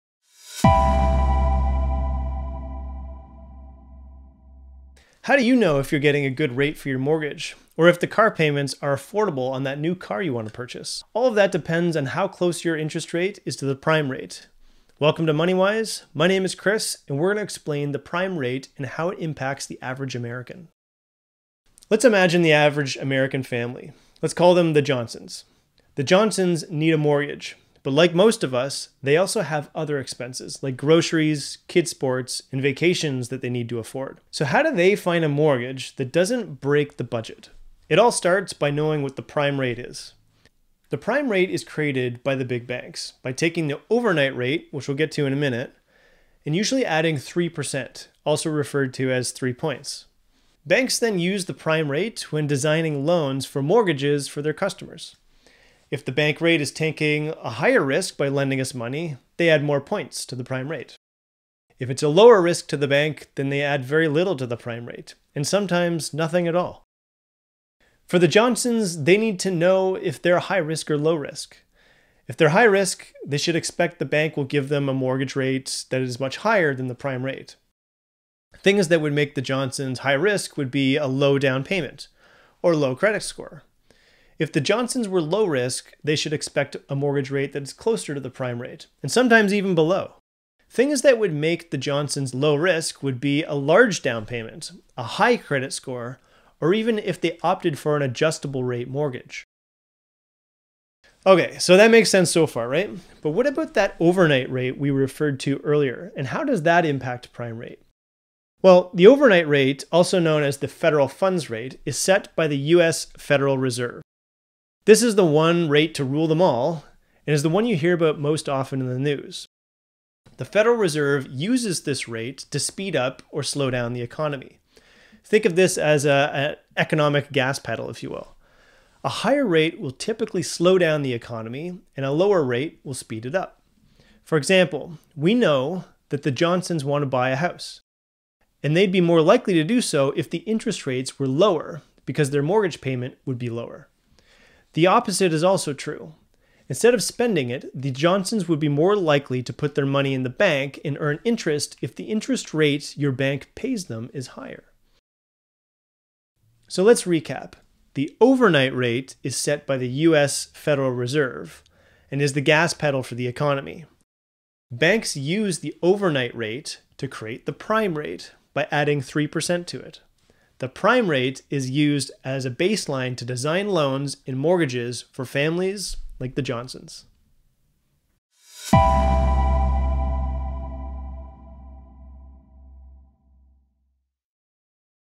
What Is The Prime Rate?
How do you know if you are getting a good rate for your mortgage, or if the car payments are affordable on that new car you want to purchase? All of that depends on how close your interest rate is to the prime rate.

The overnight rate is set by the US Federal Reserve and is the gas pedal for the economy. The banks use the overnight rate to create the prime rate by adding 3 percent to it. The prime rate is used as a baseline to design loans and mortgages for their customers.

00:00 Introduction
00:19 What is the prime rate?
01:44 What is the overnight rate?
02:35 Summary